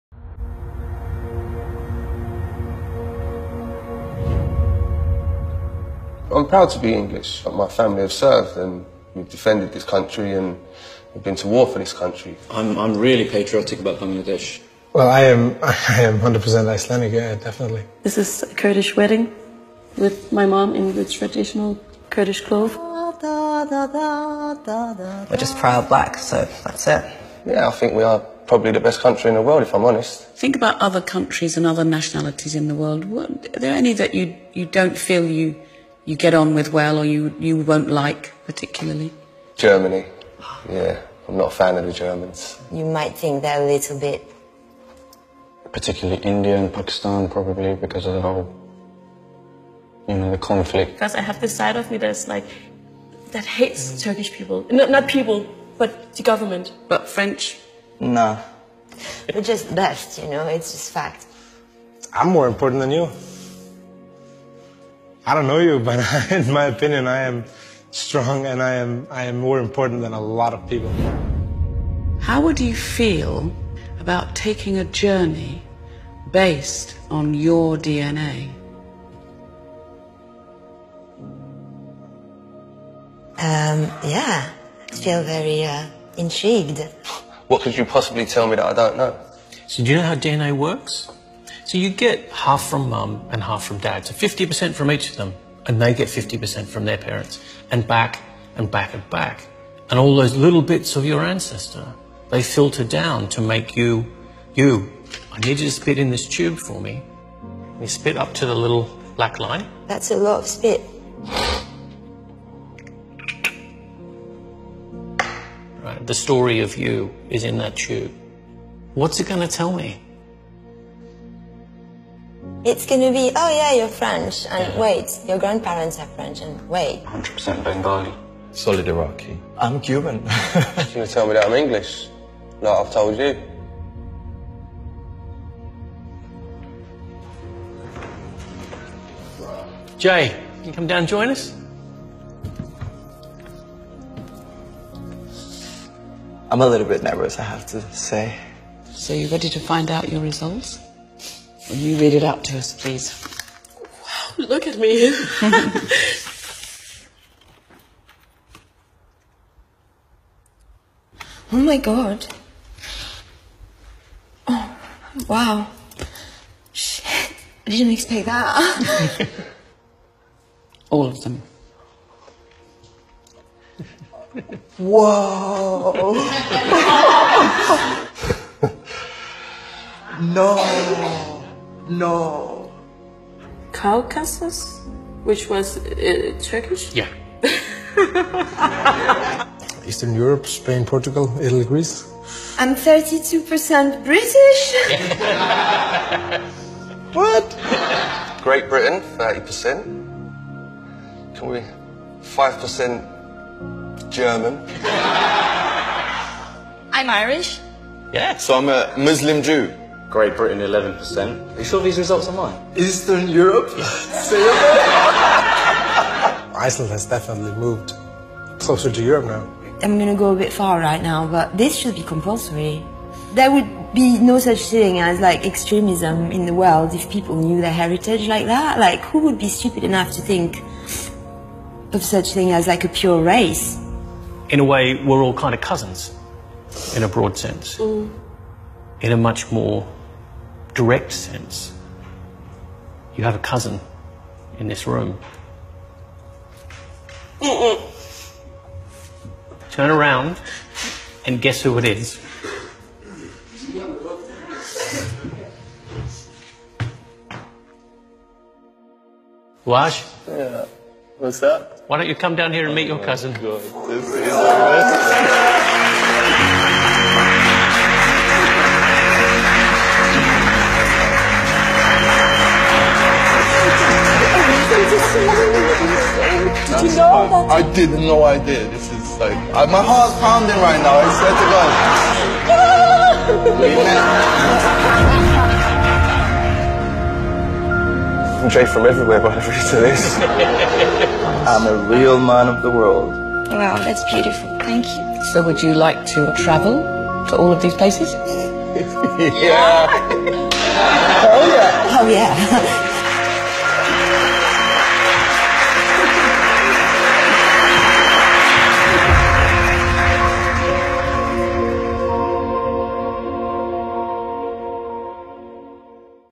all the world are relatives !! unique DNA experiment
unique DNA experiment prove that there are no  pure races ,so race is a  culture concept ..we are one family !